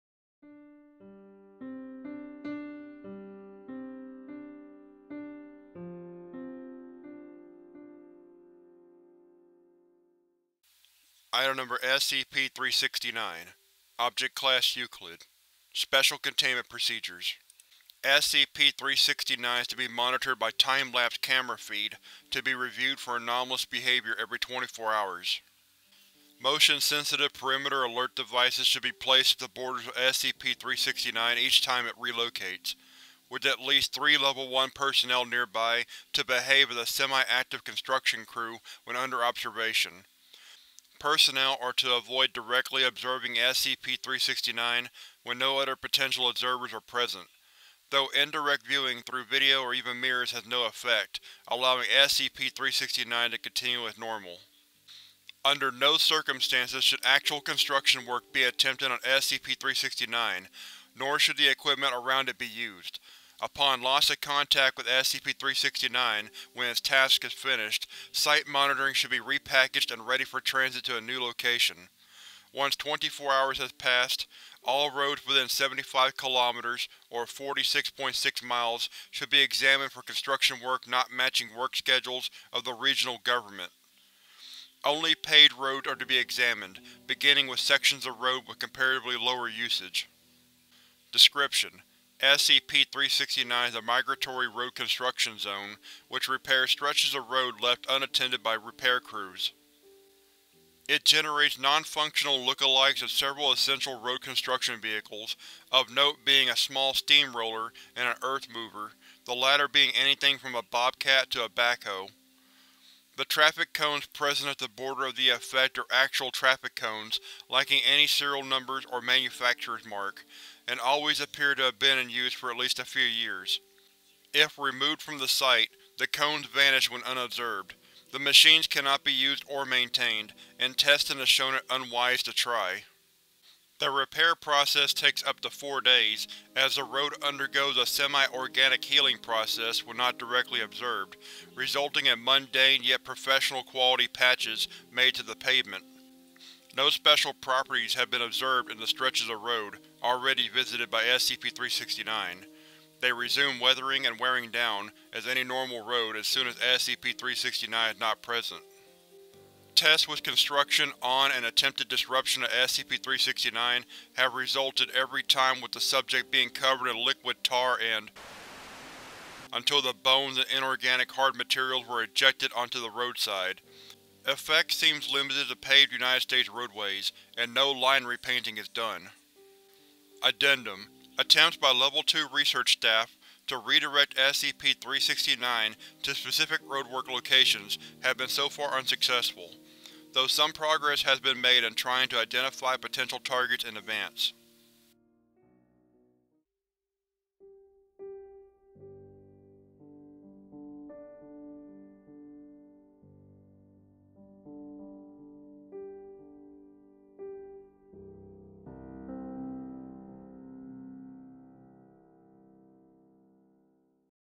SCP-369 Living Migratory Roadwork | object class Euclid
SCP-369 is a migratory road construction zone which repairs stretches of road left untended by repair crews. It generates nonfunctional look-alikes of several essential road construction vehicles, of note being a small steam-roller and an earth mover, the latter being anything from a bobcat to a backhoe. The traffic cones present at the border of the effect are actual traffic cones, lacking any serial number or manufacturer’s mark, and always appear to have been in use for at least a few years. If removed from the site, the cones vanish when unobserved. The machines cannot be used or maintained, and testing has shown it unwise to try.